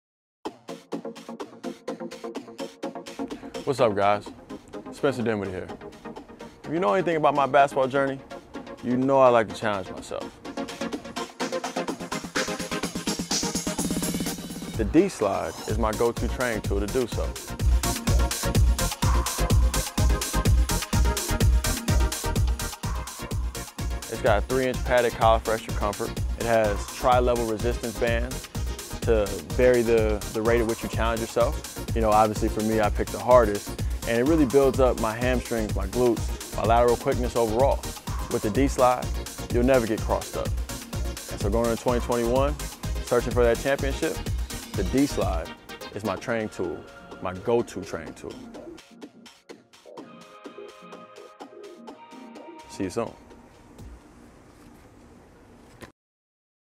Spencer Dinwiddie - D-Slide Basketball Training Device
The D-Slide® is a basketball training device that helps athletes develop lateral quickness and power. The unique design of the D-Slide® allows for optimum performance in the training and development of a pure defensive stance.